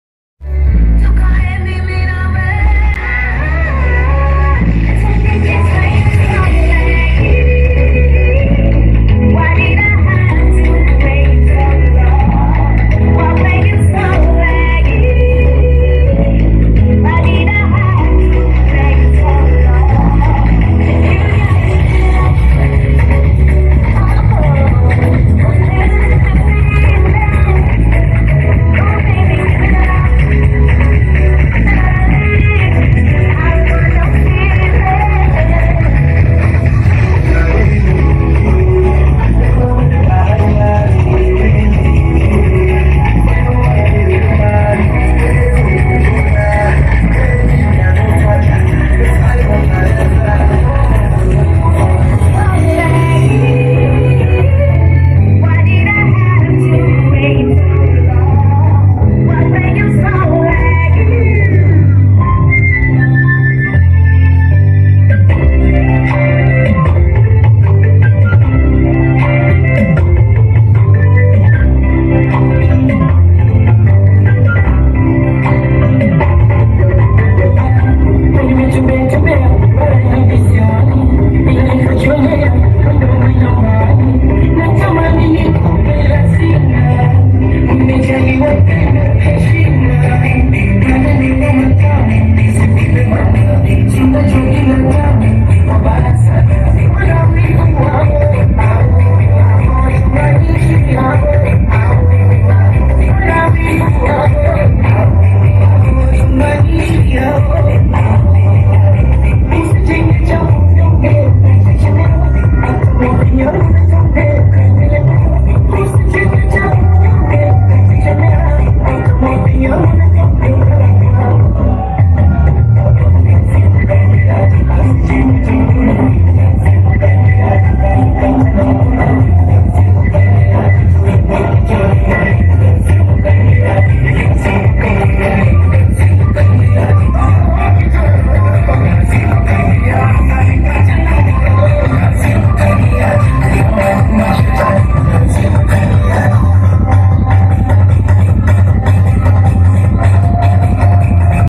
Watering Kamuzu Stadium 1
This is Sunday, October 11 2015 at Kamuzu Stadium. The video shows a vehicle watering the turf,  few minutes before kick-off of the game between Flames and Tanzania's Taifa Stars.